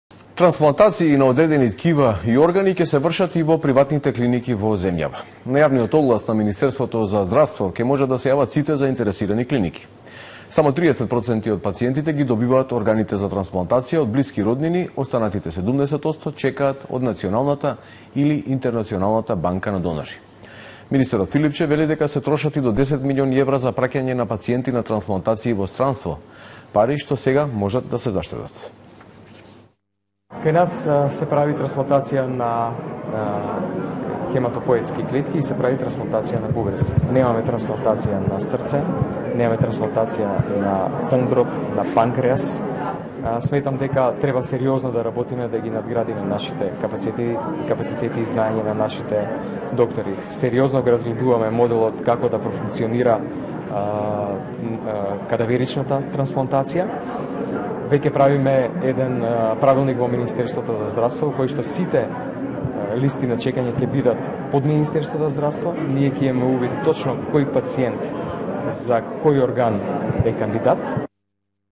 Трансплантација на органи и во приватните клиники
Трансплантации на одредени ткива и органи ќе се вршат и во приватните клиники во земјава. На јавниот оглас на Министерството за здравство ќе можат да се јават сите заинтересирани клиники.

Само 30% од пациентите ги добиваат органите за трансплантација од блиски роднини, останатите 70% чекаат од националната или интернационалната банка на донори. Министерот Филипче вели дека се трошат и до 10 милиони евра за праќање на пациенти на трансплантации во странство, пари што сега можат да се заштедат.

„Кај нас се прави трансплантација на хематопоетски клетки и се прави трансплантација на бубрези. Немаме трансплантација на срце, немаме трансплантација на црн дроб, на панкреас. Сметам дека треба сериозно да работиме да ги надградиме нашите капацитети и знаења на нашите доктори“, изјави Филипче.